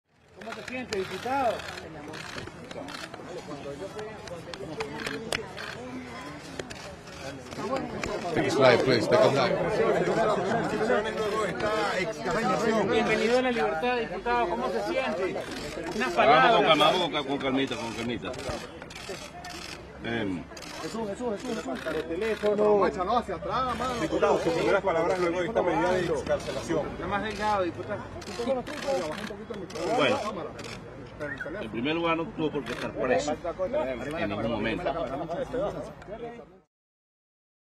Venezuela releases key opposition figure Zambrano from jail | AFP
Venezuelan key opposition figure Edgar Zambrano, the vice president of the National Assembly, appears to talk to the press after being released from jail. IMAGES